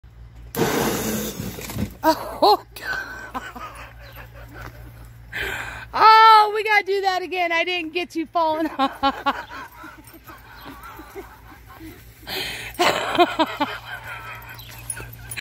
Kinda riding 76hp gocart
76hp electric gocart nearly finished. Decided to try and ride it, kinda. Went a little wrong.